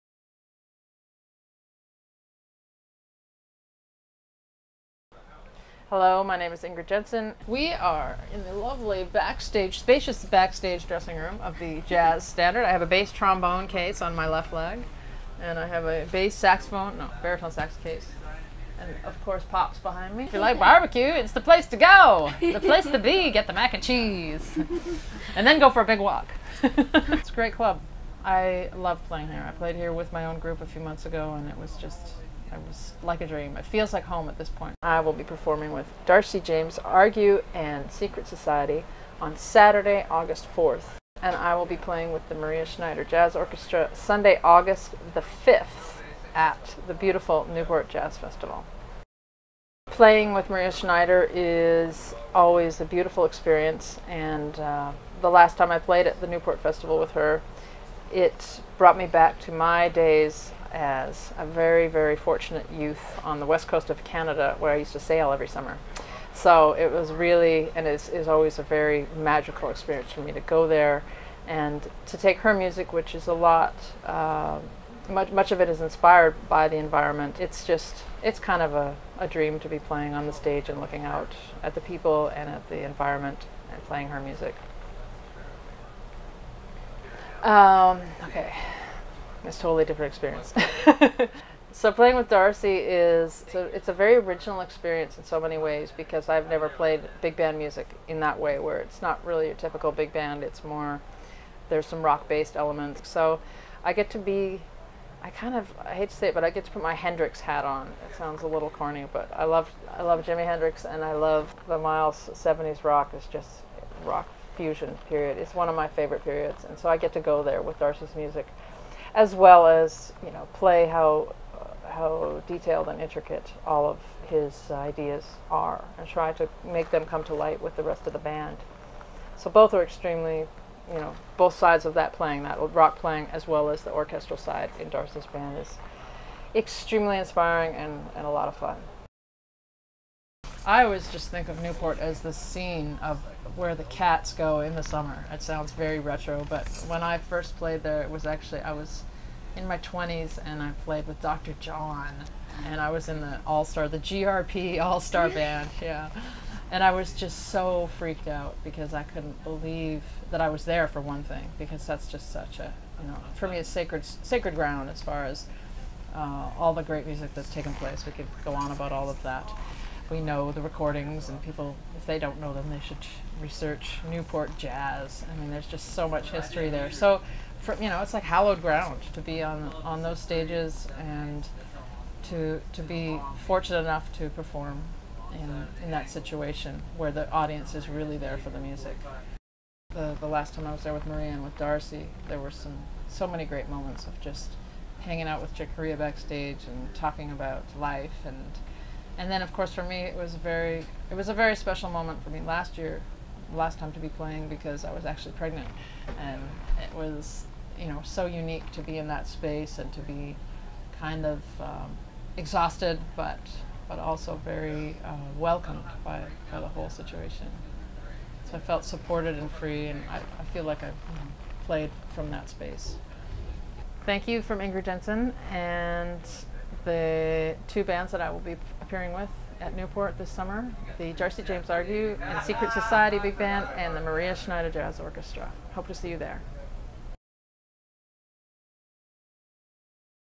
Ingrid Jensen: Newport Jazz Festival Artist Insights
Ingrid Jensen will be with Darcy James Argue Secret Society and Maria Schneider Orchestra at the 2012 Newport Jazz Festival, which will take place on August 3, 4 & 5, in Newport, Rhode Island. In this official Newport Jazz interview from Jazz Standard in New York City hear what Ingrid has to say about the festival the groups she will be with in August, Jimi Hendrix, Miles Davis, Dr. John and how they're all connected.    Ingrid will also be a part of the 2nd annual Newport Festivals Foundation gala on the evening of Saturday, August, 4 at Marble House in Newport, Rhode Island, following the festival day events.  Photos by Ayano Hisa, Doug Mason, Michael Weintrob, Lindsay Beyerstin and Walter Popp.  Produced by Underbunny.